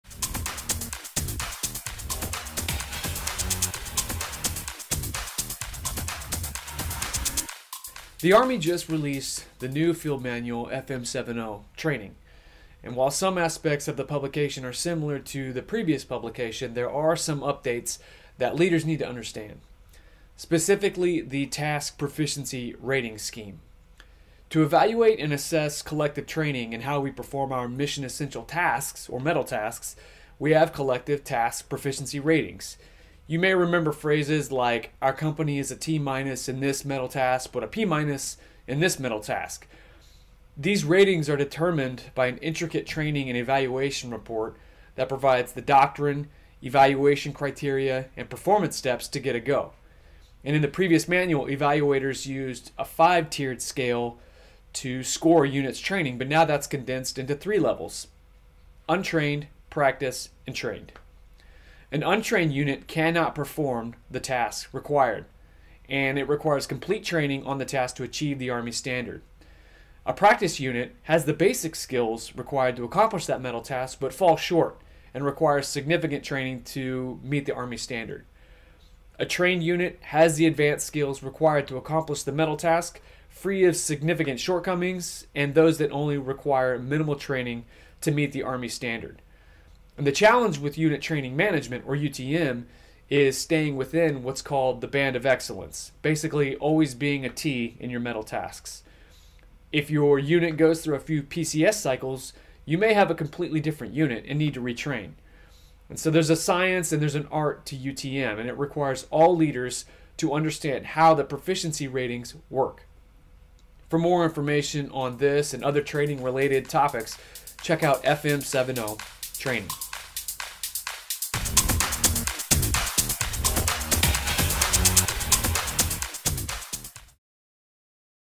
Training Proficiency
The new FM 7-0 changes collective task proficiency ratings. Do you know the differences? Check out this episode of Doctrine Digest and watch along as CPT Wyatt Harper explains the revised ratings.